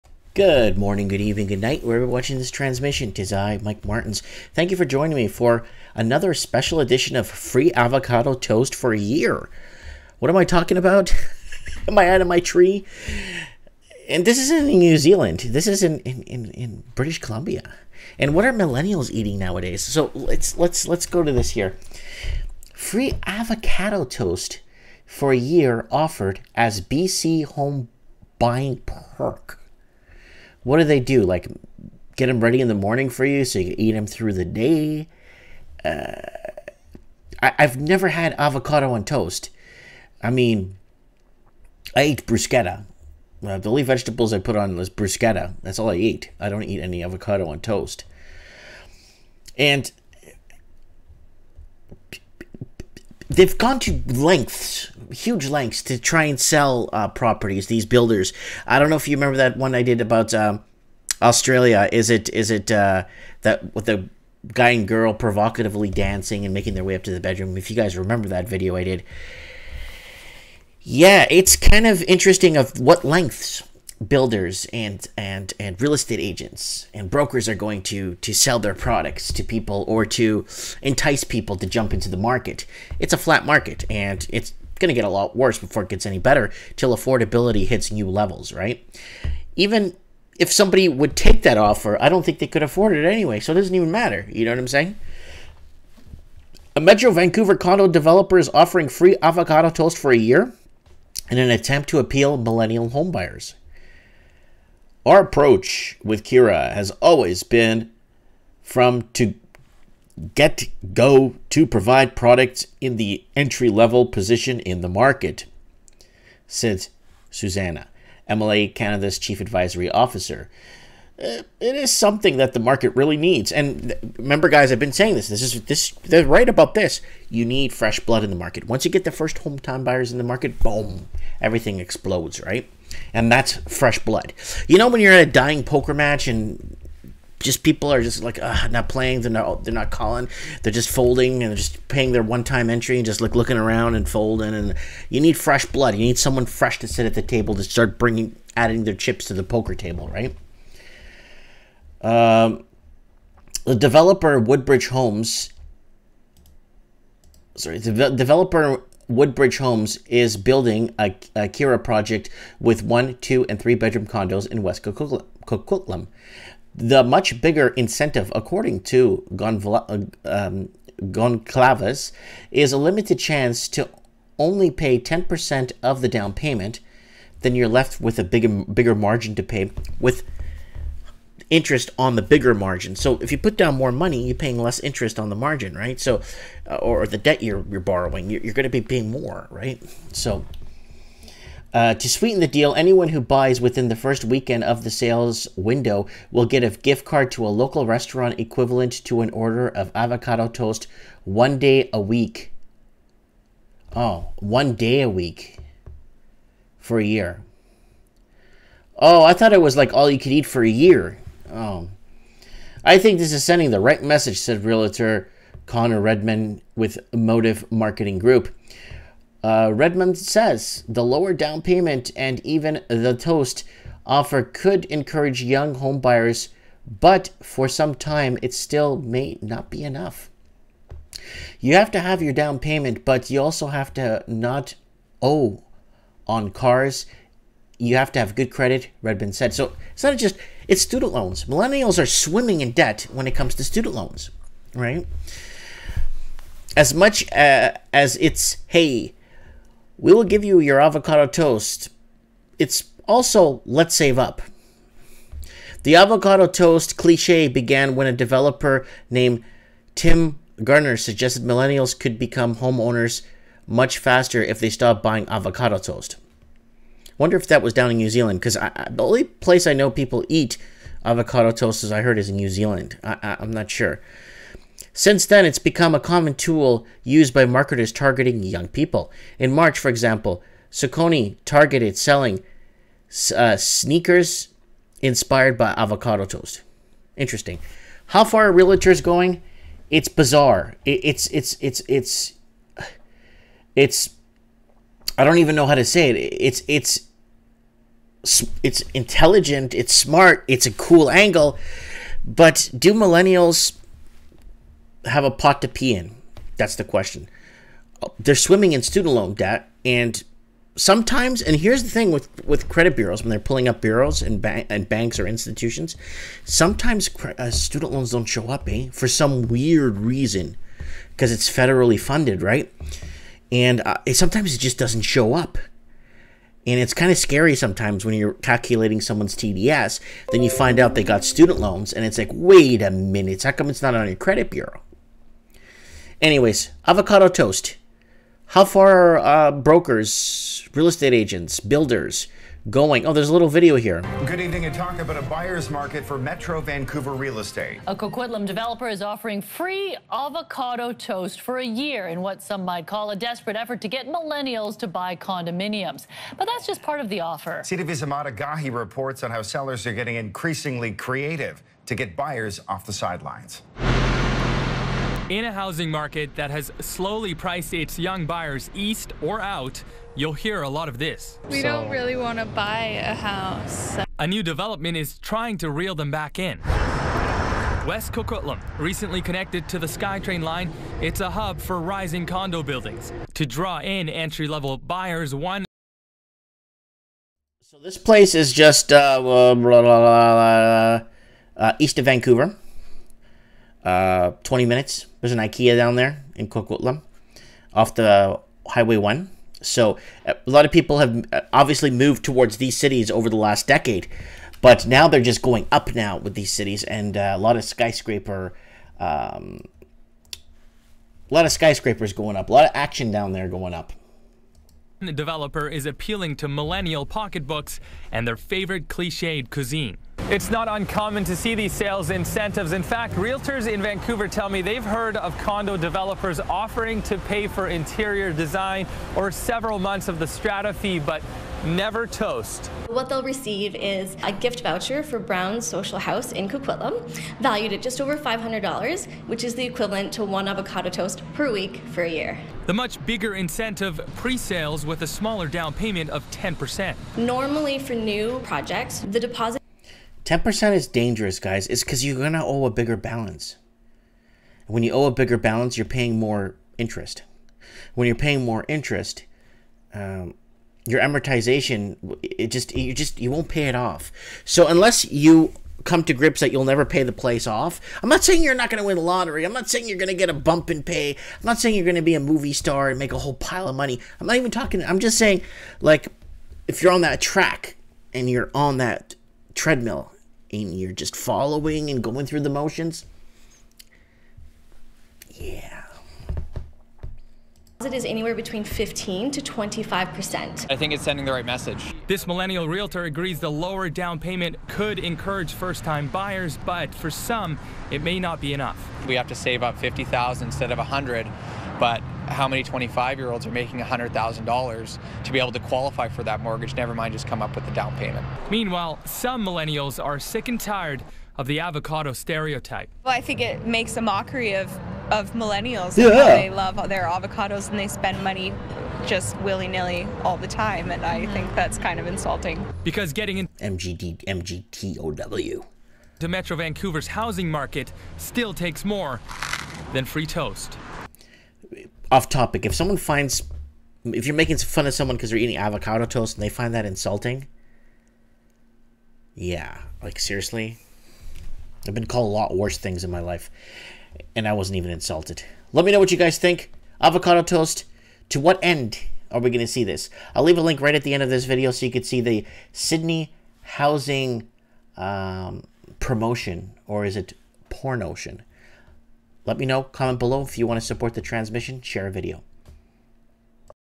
HOUSING CRISIS - Free avocado toast for a year
A Metro Vancouver condo developer is offering free avocado toast for a year – in an attempt to appeal to millennial homebuyers.

DECLAIMER - WE DO NOT OFFER OR GIVE FINANCIAL ADVICE !
AS I ALWAYS SAY - DO YOUR RESEARCH - AND ASK LICENSED PROFESSIONAL